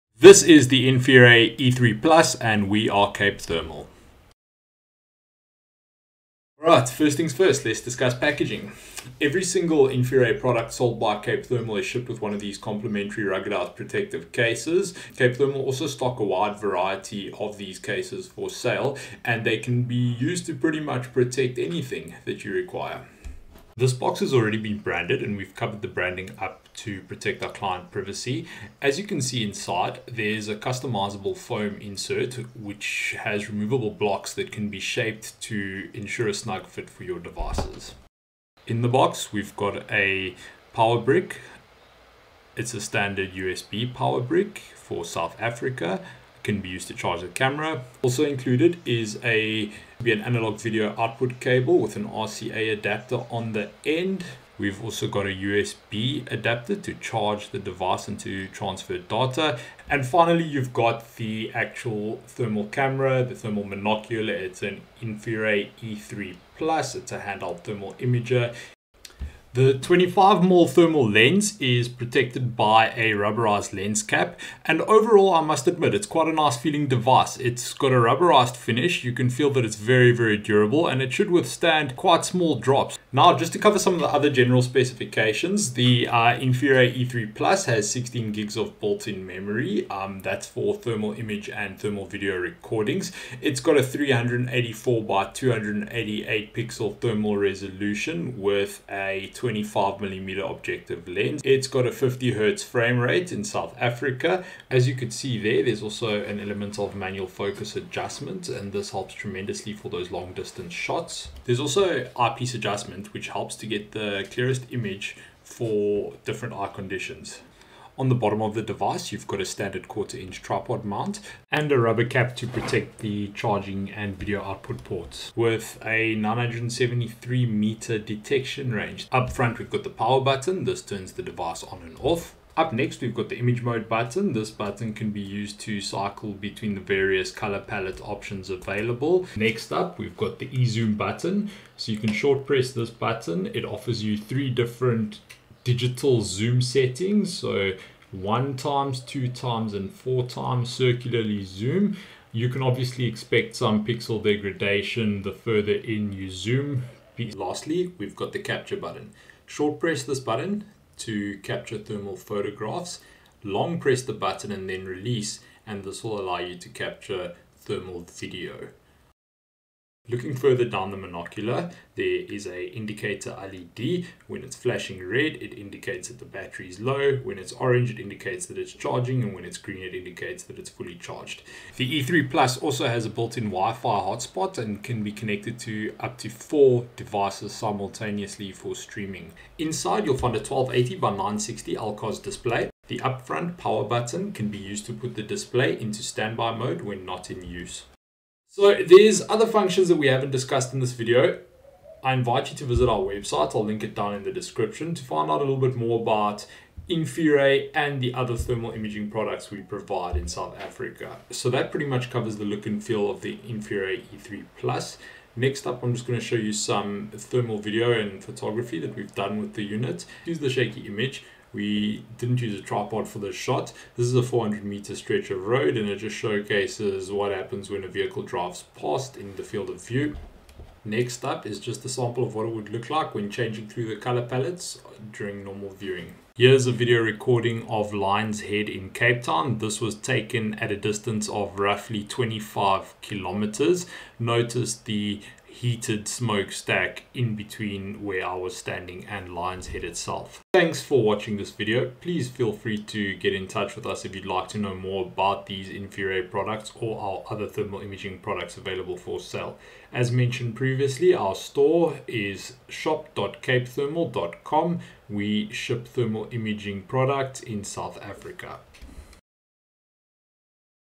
Unboxing and testing the InfiRay Eye E3 Plus V2 0 - Cape Thermal
Hey everybody!

This is the first of many videos like this. Expect major improvements in overall quality and coverage as we go.

This is an unboxing video of the E3 Plus V2.0 InfiRay thermal imaging monocular as shipped from Cape Thermal. We test the overall recording quality as part of the video.

Our ruggedized storage collection (P.S - we include one of these for free when you buy InfiRay):